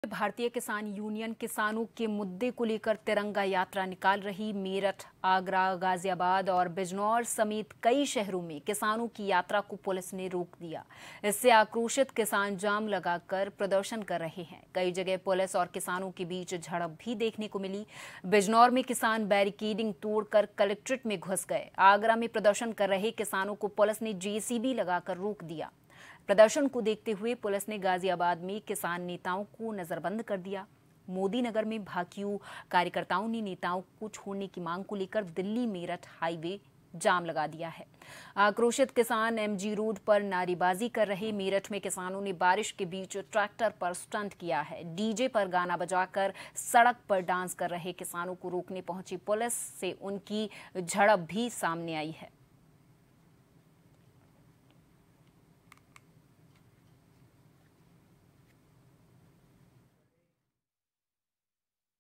Ghaziabad UP News | तिरंगा मार्च निकाल रहे किसानों को रोका, किसानों ने किया जोरदार प्रदर्शन
Watch JAN TV on :
Tata Play DTH : 1185
Airtel DTH: 355
JIO Fiber: 1384

News Credit-MKP